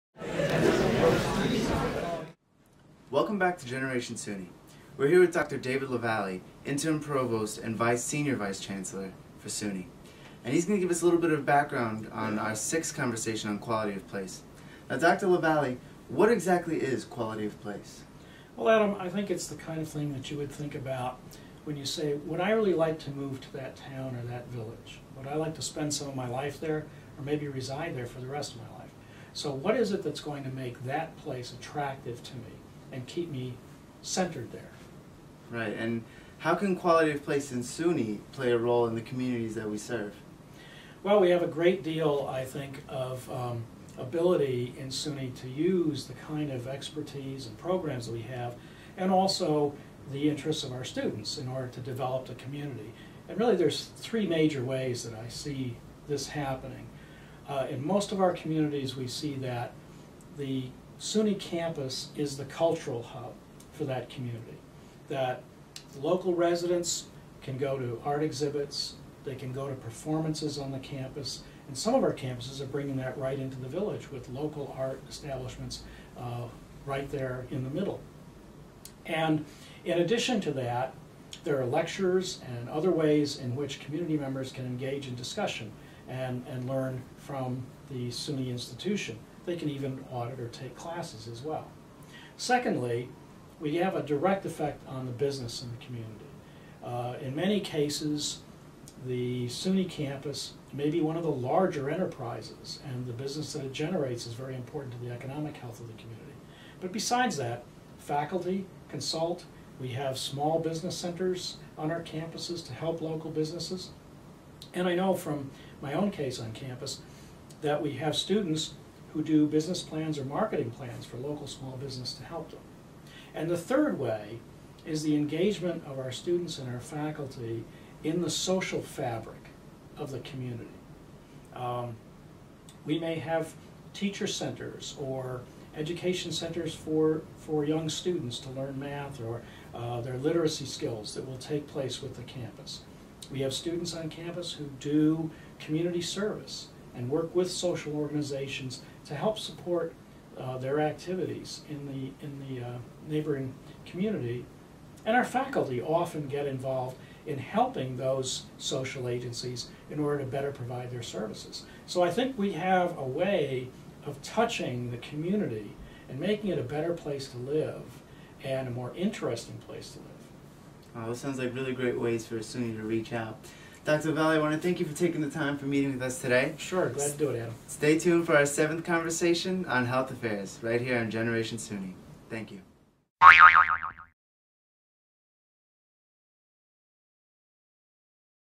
Quality of Place - David Lavallee
Dr. David K. Lavallee, Interim Senior Vice Chancellor and Provost of the State University of New York talks about the role of SUNY in improving New York's "quality of place" - and defining what that is!